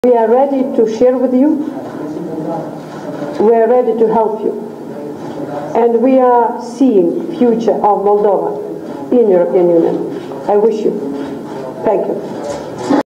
Conferință de presă susținută de președintele Republicii Moldova, Nicolae Timofti, și președintel...
Titlul evenimentului: Conferință de presă susținută de președintele Republicii Moldova, Nicolae Timofti, și președintele Republicii Lituania, Dalia Grybauskaitė